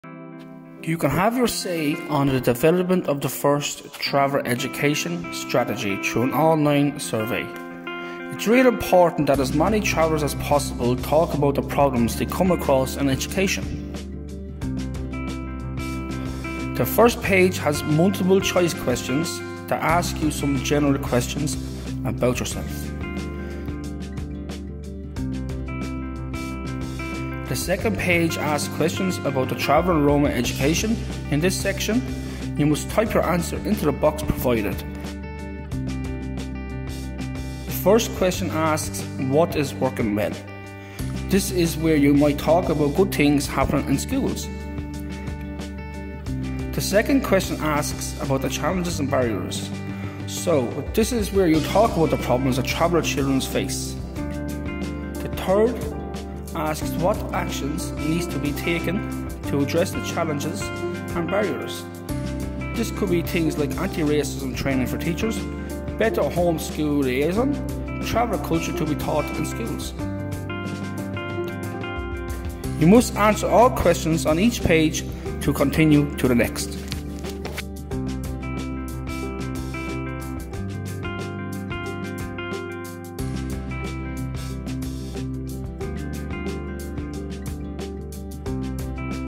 Traveller Education Strategy - Online Consultation Form
This short video explains the Department of Education Online Consultation for the Traveller Education Strategy.  The form is live during October 2023.  Other consultations on this long-awaited Strategy are also due to take place.